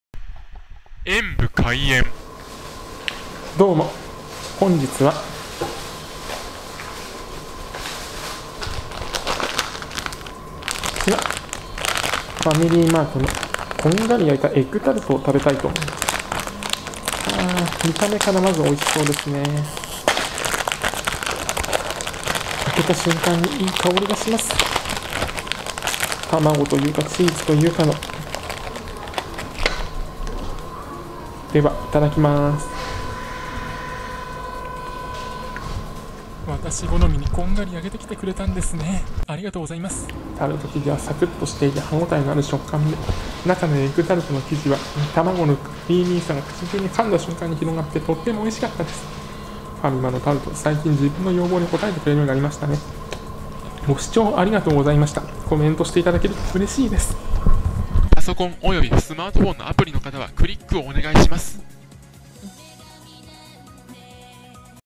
ファミリーマート こんがり焼いたエッグタルトが私好みに夏らしく焼けてきてくれた～
ファミリーマートのエッグタルトが夏らしく日焼けしてきたみたいです。で、そのこんがり焼いた生地のお味は～
贈り物は此方まで↓
〒153-0042